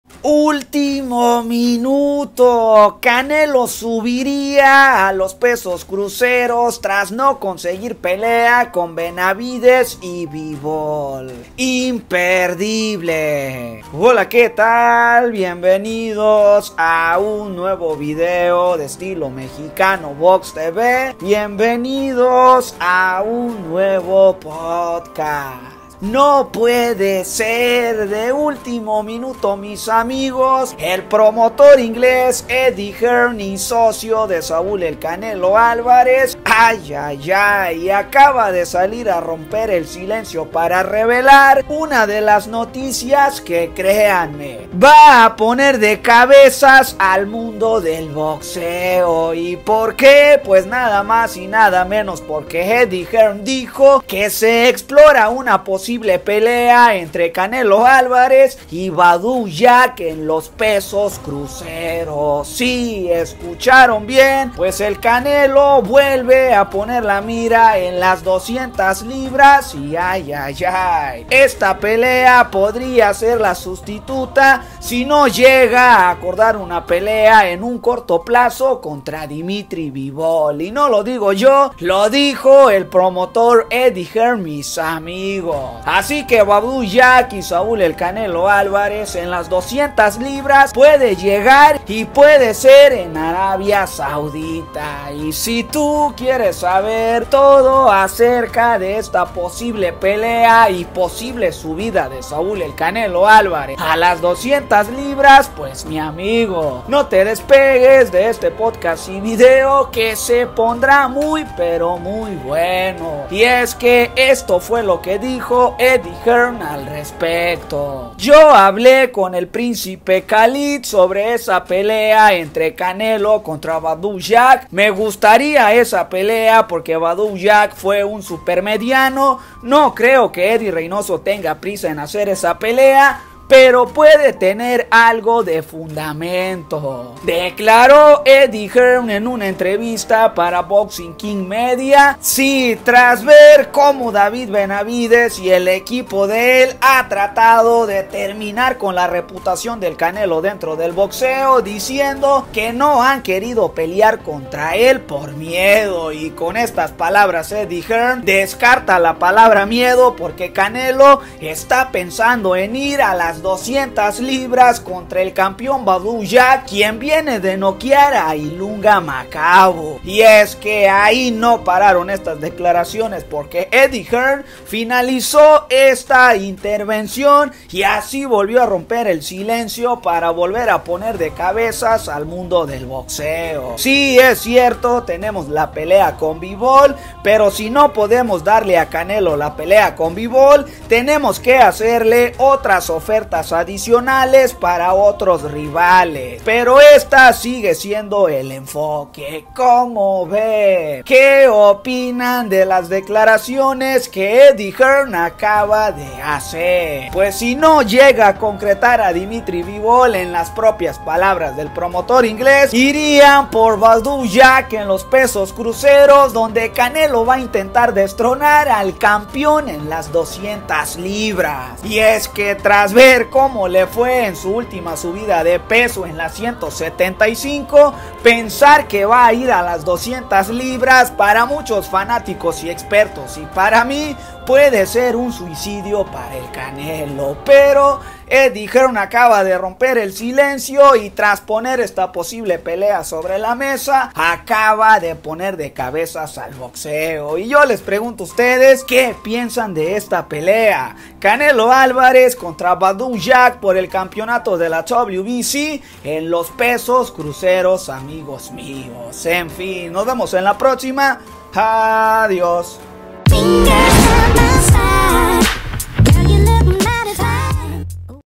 ¡ÚLTIMO MINUTO! Canelo SUBIRÍA a los PESOS CRUCEROS tras no conseguir pelea con Bivol / Podcast
Canelo SUBE A LAS 200 LIBRAS‼️